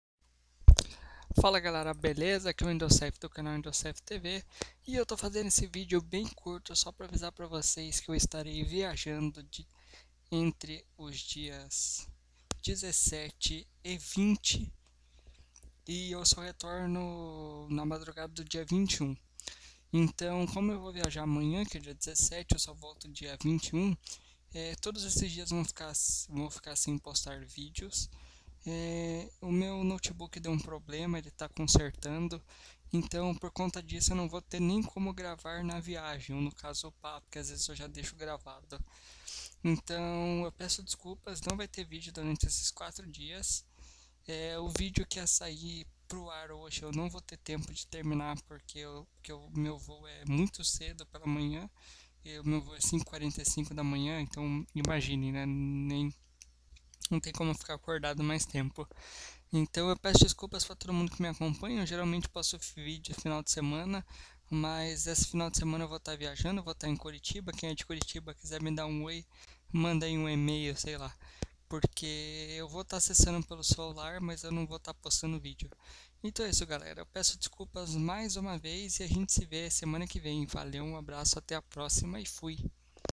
Não haverá vídeos!
Sim vou viajar por 4 dias, vejo vocês na volta!